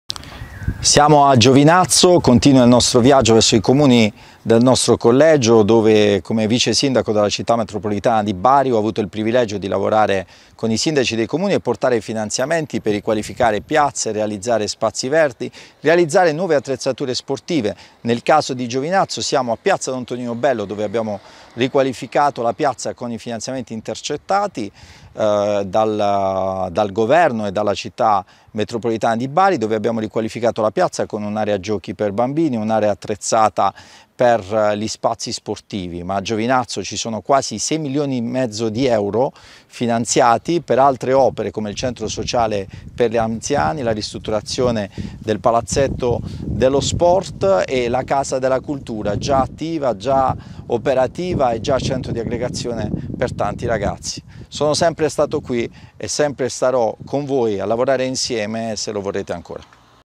Il candidato alla Camera Michele Abbaticchio e i finanziamenti intercettati per Giovinazzo
Abbaticchio è candidato nel collegio uninominale Puglia 4 alla Camera dei Deputati. Il collegio comprende i comuni di Adelfia, Binetto, Bitetto, Bitonto, Casamassima, Conversano, Corato, Giovinazzo, Molfetta, Monopoli, Palo del Colle, Polignano a Mare, Rutigliano, Ruvo di Puglia, Sannicandro di Bari, Terlizzi e Turi.